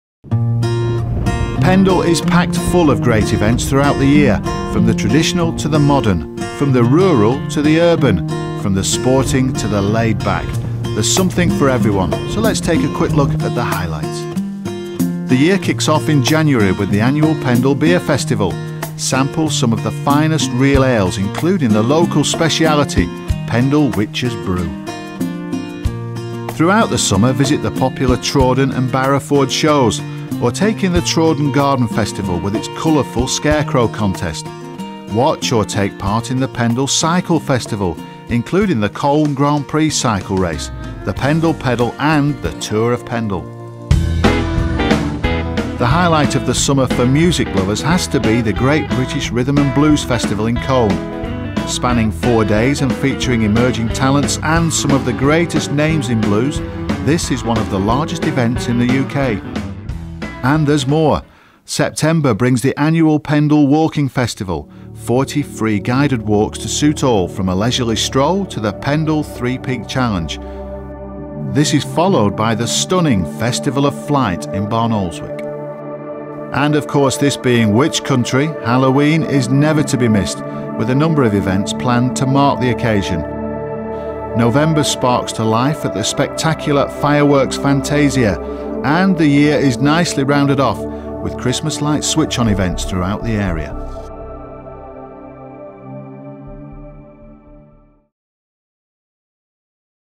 events FL9 Widescreen 1280x720
Pendle is an area of stunning landscapes in Lancashire, Northern England. Climb Pendle Hill for sheer inspiration, or walk the Pendle Way, or try the famous Witches Trail. There are also loads of great cycle and walking routes, and great events all year round, like Pendle Walking Festival and the Great British Rythm & Blues Festival in Colne. Recognise the voiceover? Its Pendle's own local boy, BBC Radio & TV presenter, Tony Livesey.